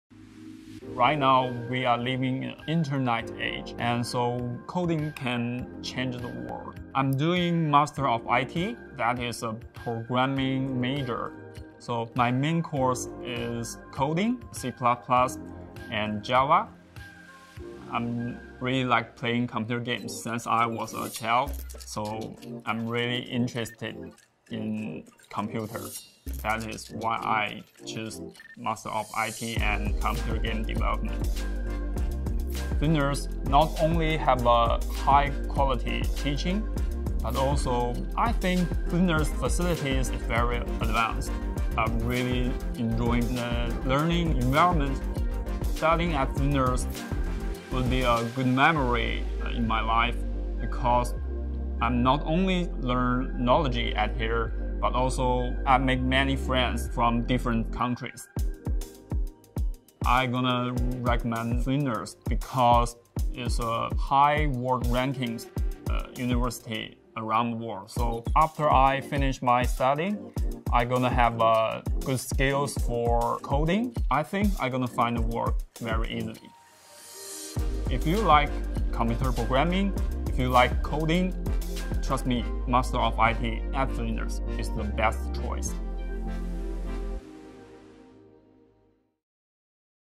Change the world with a Flinders Master of IT
"Right now we are living in an internet age, so coding can change the world," says Flinders Master of IT student Shan Gao.

"I really liked playing computer games since I was a child, so I am really interested in computers. That is why I chose Master of IT and Computer Game Development."

If, like Shan, you have a love of computers, gaming and coding, discover why a Flinders Master of IT could be the best choice to ensure your career is future ready.